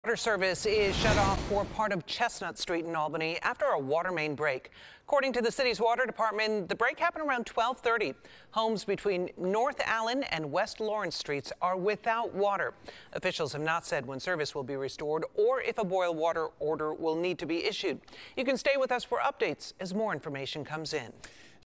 Water main break in Albany
The Albany Water Department announced that crews are currently on the scene of a water main break.